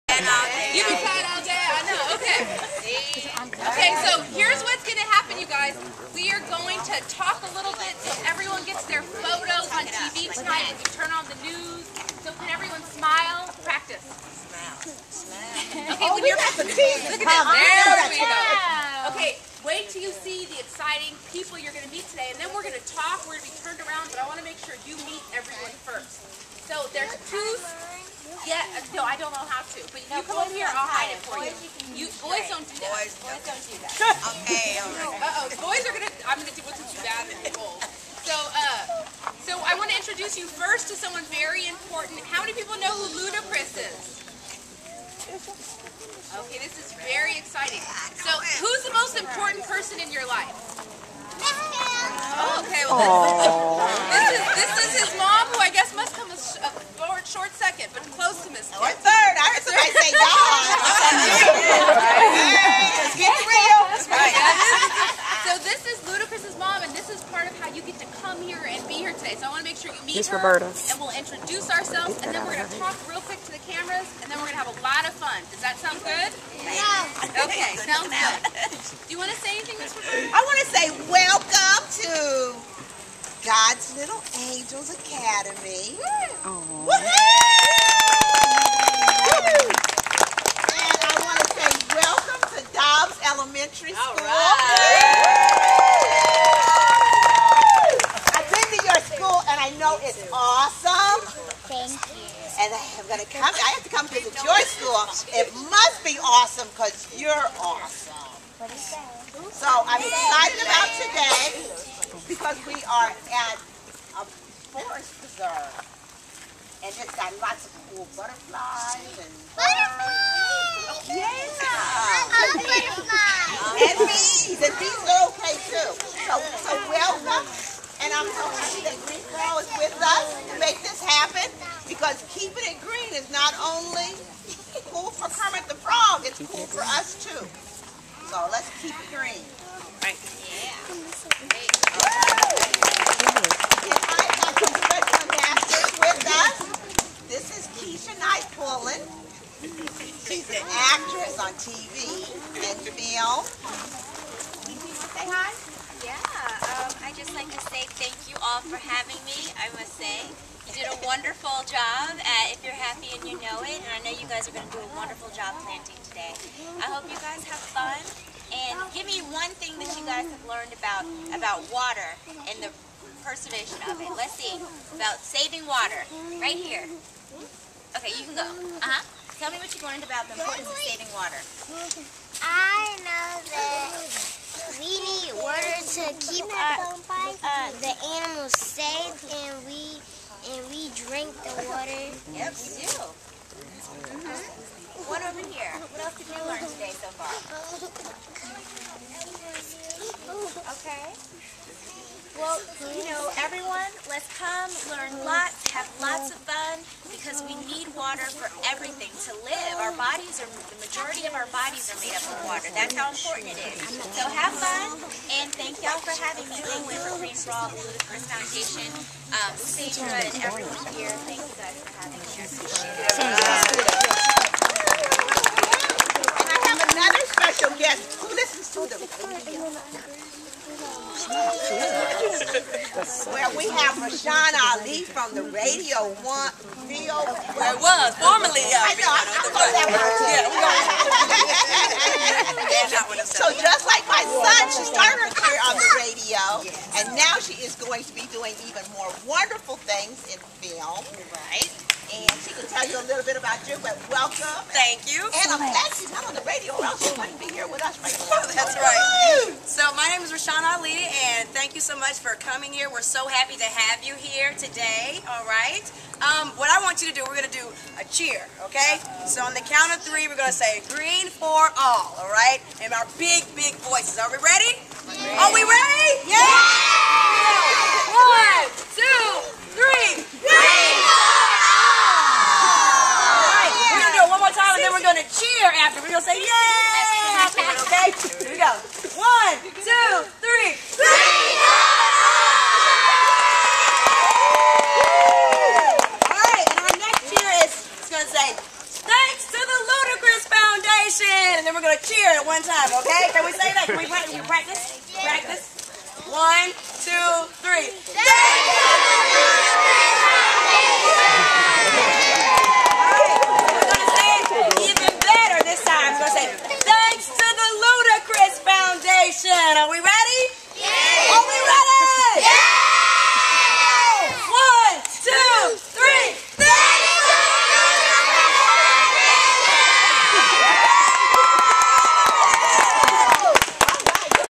Earth Day 2011
Earth Day Celebration at the Outdoor Activity Center, 1442 Richland Rd, SW, Atlanta, Ga 30310.